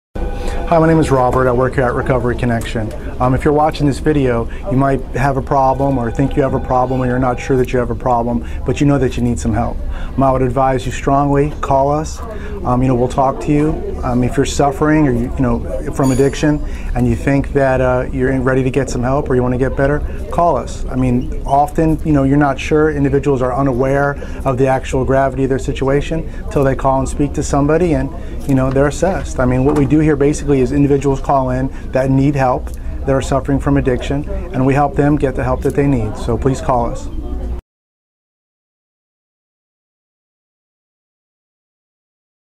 Recovery Connection Can Provide Addiction Treatment Help
If you or your loved one is struggling from drug or alcohol addiction and think you may need help, the Recovery Connection helpline can assist you in finding the addiction treatment you need. A brief phone conversation with one of our addiction specialists can direct you to a recovery facility that is right for you.